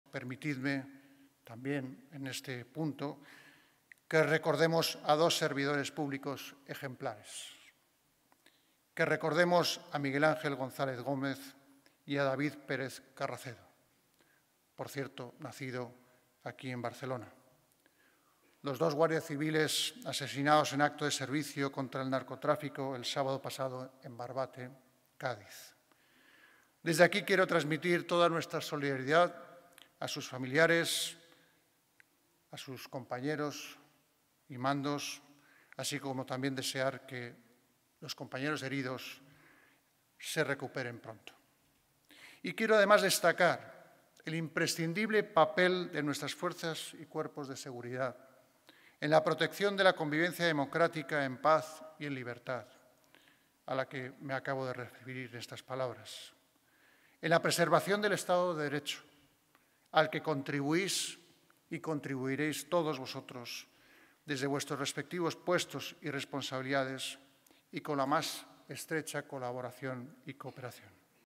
El Rey recuerda a los guardias civiles asesinados en acto de servicio contra el narcotráfico
El Rey ha definido como servidores públicos ejemplares a los dos guardias civiles "asesinados en acto de servicio contra el narcotráfico" el sábado en Barbate (Cádiz).

   Lo ha dicho este martes durante su discurso en la entrega de despachos a los jueces de la LXXII Promoción de la Carrera Judicial, que ha presidido en Barcelona.

   Felipe VI ha dedicado un recuerdo a los fallecidos al hacer referencia a que "la sociedad debe ser plenamente consciente" de la aportación de los jueces en particular y de los servidores públicos en general.